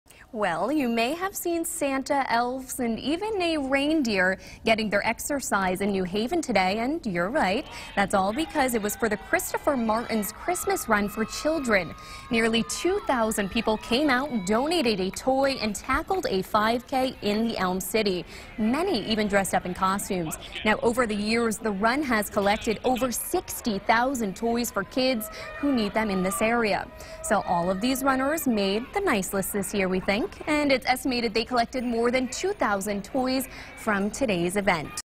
Santas sprint through New Haven during Christopher Martin's Christmas Run
You might have seen Santa, elves, and even a reindeer getting their exercise in New Haven on Sunday.